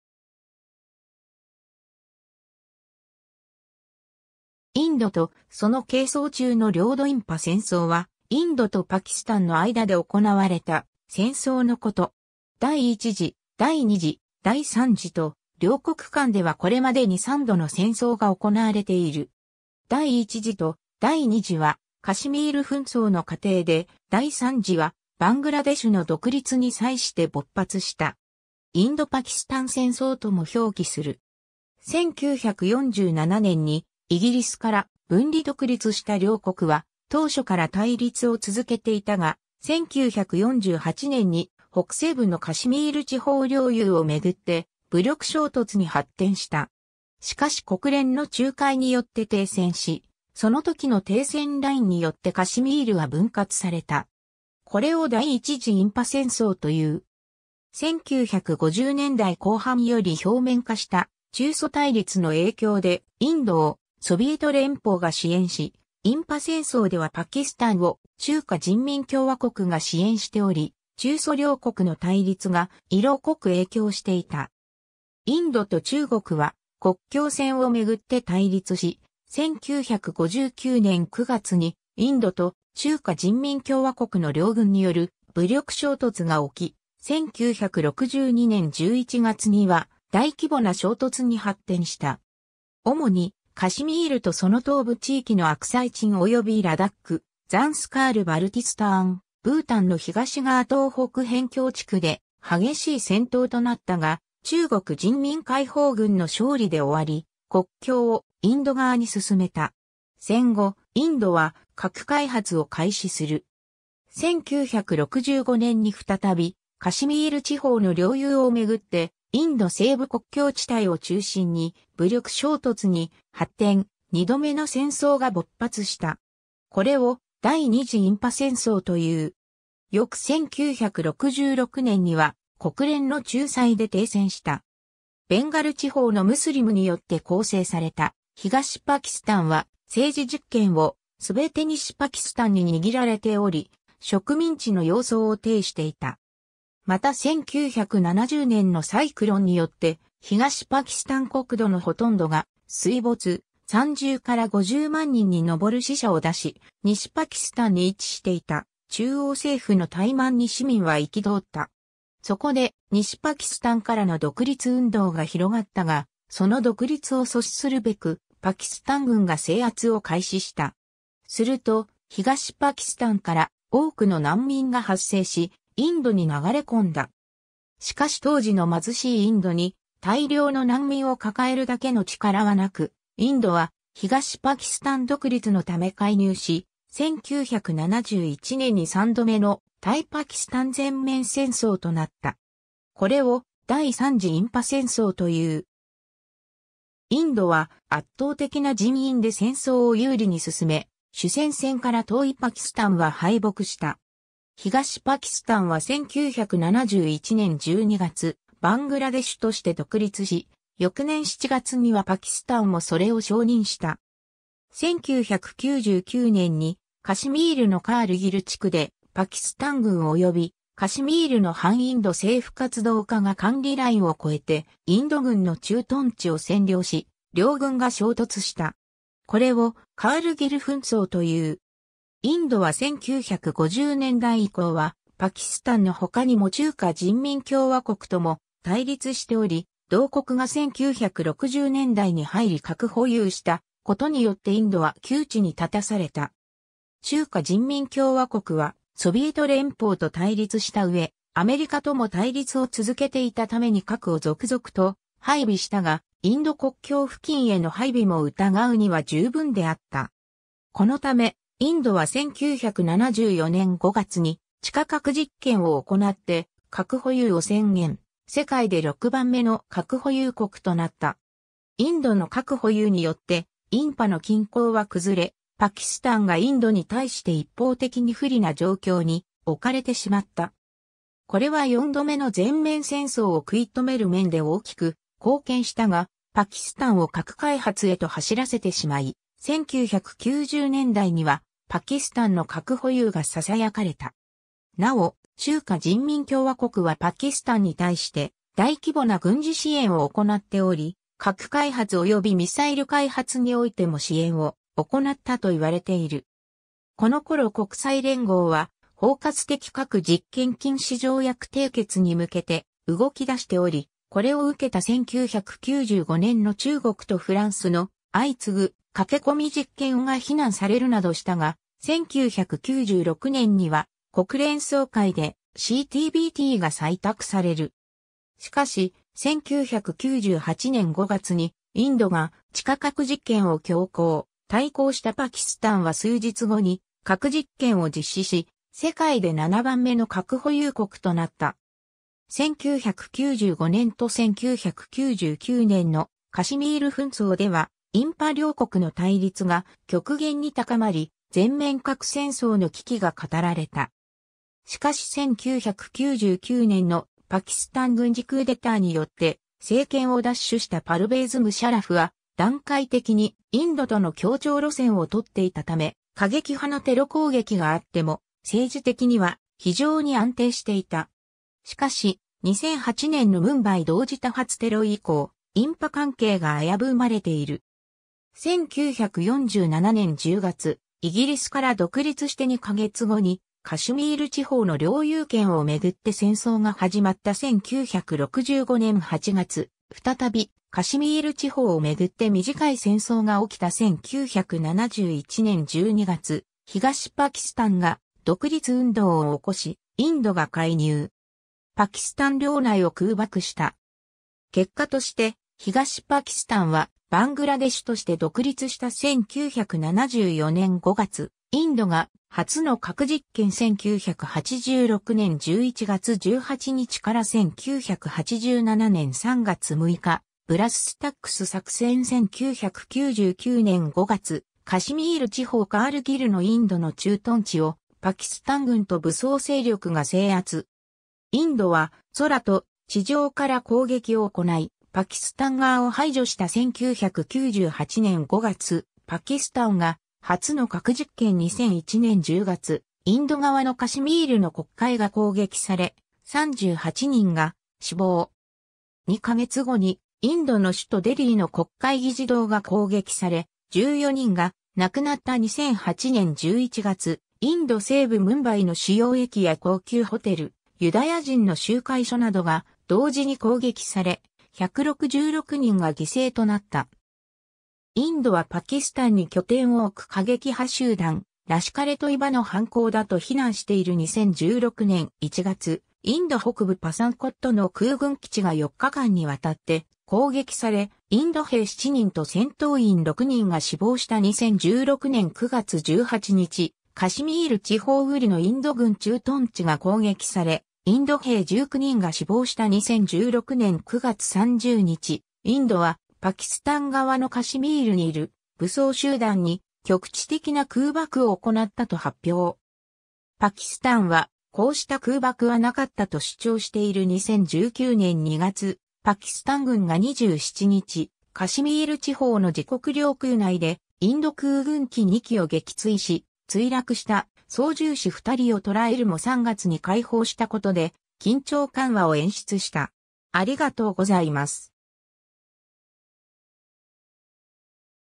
印パ戦争
インド・パキスタン関係
インドとその係争中の領土 印パ戦争（いんパせんそう）は、インドとパキスタンの間で行なわれた戦争のこと。
第一次（1947年）、第二次（1965年）、第三次（1971年）と、両国間ではこれまでに3度の戦争が行われている。
第一次と第二次はカシミール紛争の過程で、第三次はバングラデシュの独立に際して勃発した。
インド・パキスタン戦争とも表記する。
 1947年にイギリスから分離独立した両国は当初から対立を続けていたが、1948年に北西部のカシミール地方領有をめぐって武力衝突に発展した。
しかし国連の仲介によって停戦し、そのときの停戦ラインによってカシミールは分割された。
これを第一次印パ戦争という。
 1950年代後半より表面化した中ソ対立の影響で、インドをソビエト連邦が支援し、印パ戦争ではパキスタンを中華人民共和国が支援しており、中ソ両国の対立が色濃く影響していた。
インドと中国は国境線をめぐって対立し、1959年9月にインドと中華人民共和国の両軍による武力衝突が起き、1962年11月には大規模な衝突に発展した。
主にカシミールとその東部地域のアクサイチンおよびラダック・ザンスカール・バルティスターン、ブータンの東側東北辺境地区（現在のアルナーチャル・プラデーシュ州）で激しい戦闘となったが、中国人民解放軍の勝利で終わり、国境をインド側に進めた。
戦後、インドは核開発を開始する。
 1965年に再びカシミール地方の領有をめぐって、インド西部国境地帯を中心に武力衝突に発展、二度目の戦争が勃発した。
これを第二次印パ戦争という。
翌1966年には国連の仲裁で停戦した。
 ベンガル地方のムスリムによって構成された東パキスタンは、政治実権を全て西パキスタン（現在のパキスタン）に握られており、植民地の様相を呈していた。
また1970年のサイクロンによって東パキスタン国土の殆どが水没、30 - 50万人に上る死者を出し、西パキスタンに位置していた中央政府の怠慢に市民は憤った。
 そこで西パキスタンからの独立運動が広がったが、その独立を阻止するべく、パキスタン軍が制圧を開始した。
すると東パキスタンから多くの難民が発生し、インドに流れ込んだ。
しかし当時の貧しいインドに大量の難民を抱えるだけの力はなく、インドは東パキスタン独立のため介入し、1971年に3度目の対パキスタン全面戦争となった。
これを第三次印パ戦争という。
インドは圧倒的な人員で戦争を有利に進め、主戦線から遠いパキスタンは敗北した。
東パキスタンは1971年12月、バングラデシュとして独立し、翌年7月には（西）パキスタンもそれを承認した。
 1999年に、カシミールのカールギル地区でパキスタン軍およびカシミールの反インド政府活動家が管理ライン（停戦ライン）を超えてインド軍の駐屯地を占領し、両軍が衝突した。
これをカールギル紛争という。
 インドは1950年代以降は、パキスタンの他にも中華人民共和国とも対立しており、同国が1960年代に入り核保有したことによってインドは窮地に立たされた。
中華人民共和国はソビエト連邦と対立した上、アメリカとも対立を続けていたために核を続々と配備したが、インド国境付近への配備も疑うには十分であった。
このため、インドは1974年5月に地下核実験を行って核保有を宣言、世界で6番目の核保有国となった。
インドの核保有によって印パの均衡は崩れ、パキスタンがインドに対して一方的に不利な状況に置かれてしまった。
 これは4度目の全面戦争を食い止める面で大きく貢献したが、パキスタンを核開発へと走らせてしまい、1990年代にはパキスタンの核保有がささやかれた。
なお、中華人民共和国はパキスタンに対して大規模な軍事支援を行っており、核開発及びミサイル開発においても支援を行ったと言われている。
 このころ国際連合は包括的核実験禁止条約（CTBT）締結に向けて動き出しており、これを受けた1995年の中国とフランスの相次ぐ「駆け込み実験」が非難されるなどしたが、1996年には国連総会でCTBTが採択される。
しかし、1998年5月にインドが地下核実験を強行、対抗したパキスタンは数日後に核実験を実施し、世界で7番目の核保有国となった。
 1995年と1999年の「カシミール紛争」では印パ両国の対立が極限に高まり、全面核戦争の危機が語られた。...